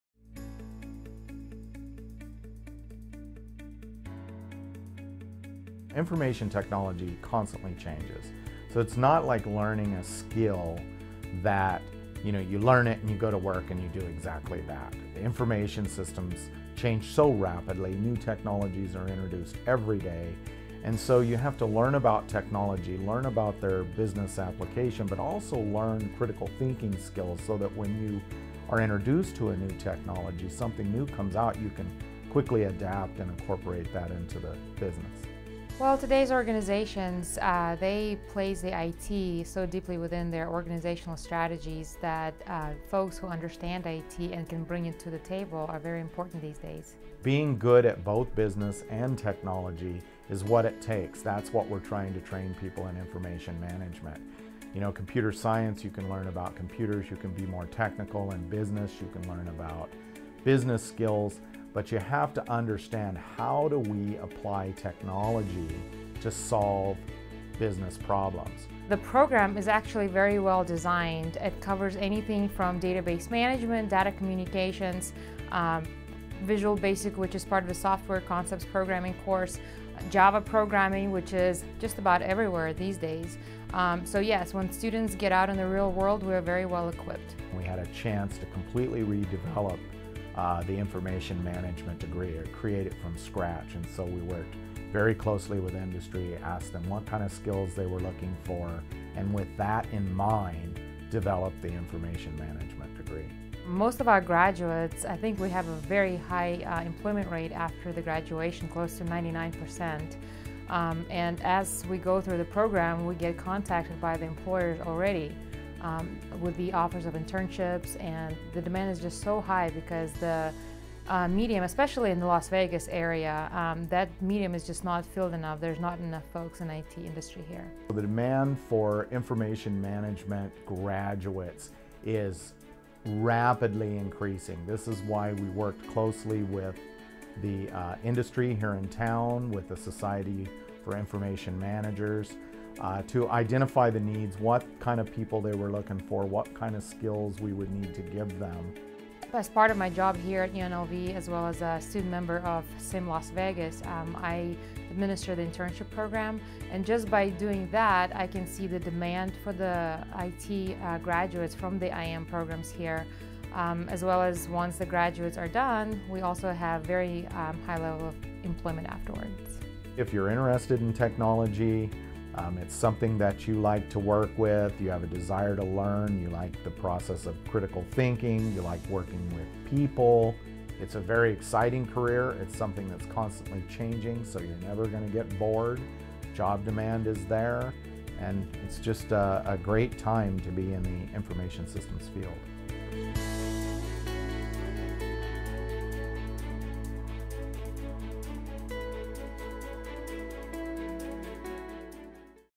UNLV | MIS Information Management Degree - Professor/Student Perspective
The Master of Science - Management Information Systems (MS MIS) program prepares graduates for professional careers in the management of information technology (IT). The MS MIS focuses on the application of technology to help achieve organizational goals and solve business problems.

This video focuses on the degree from professor and student perspectives.